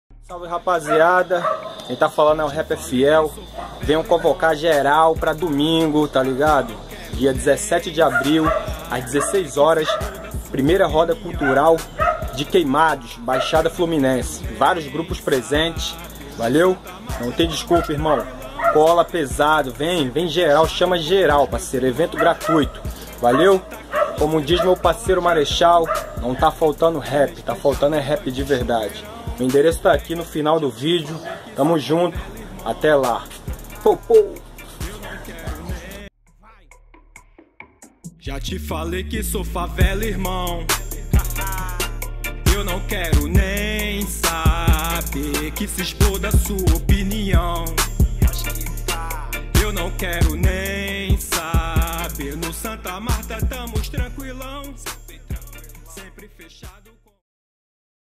Repper Fiell convoca!
Domingo 17 de Abril ás 16hs acontecerá a 1º roda cultural de Queimados na Baixada Fluminense - RJ.

Vários grupos presentes.

Praça D Branca, Próximo ao Taz Burguer.

Evento gratuito.

Até lá,

Repper Fiell.